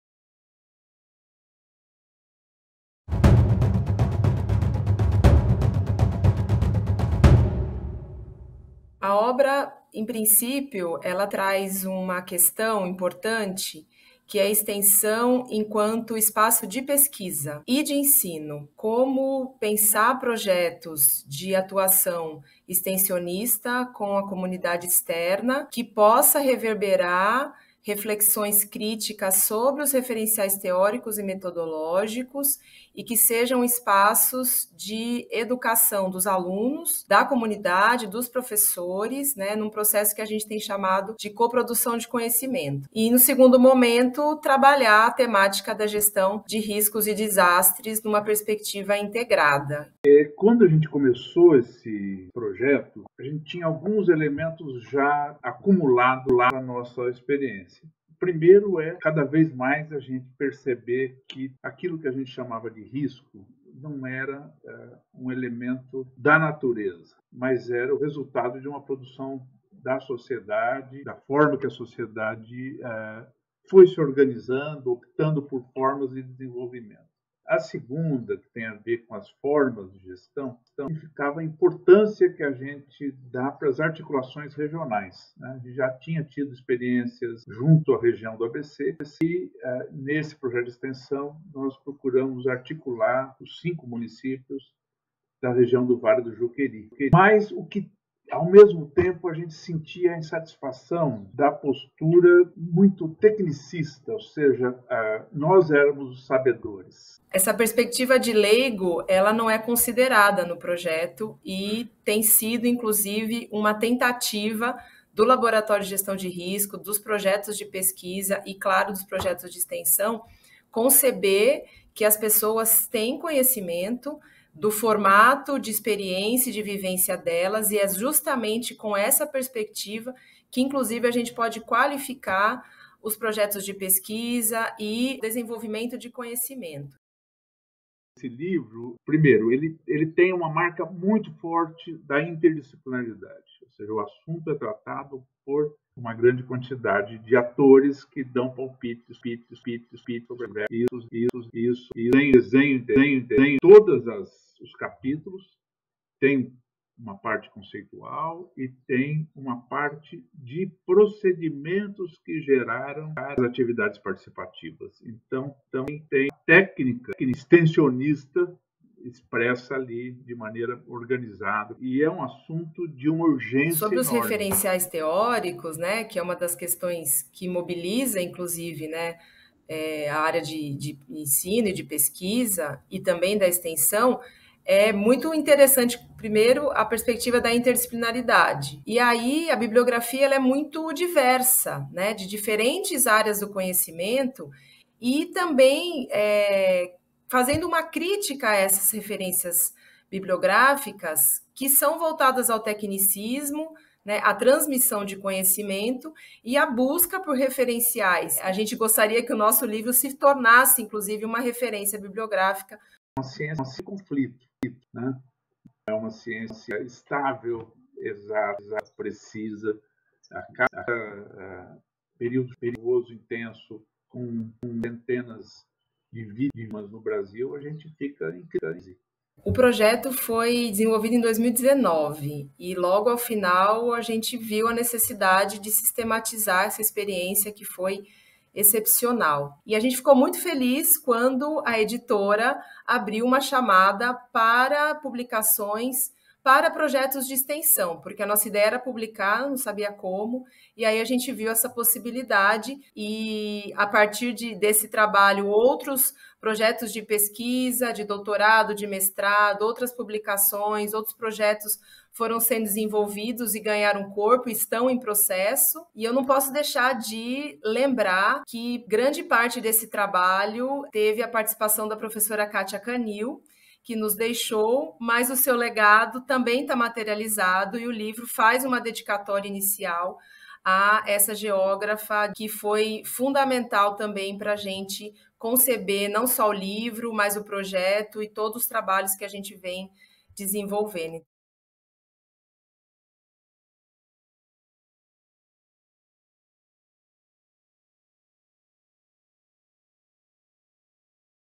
[EdUFABC] Caminhos participativos para gestão integrada de riscos e desastres
Saiba mais sobre a publicação do livro "Caminhos participativos para gestão integrada de riscos e desastres - um projeto de pesquisa e extensão universitária" nas palavras de seus organizadores, Samia Sulaiman e Fernando Rocha Nogueira.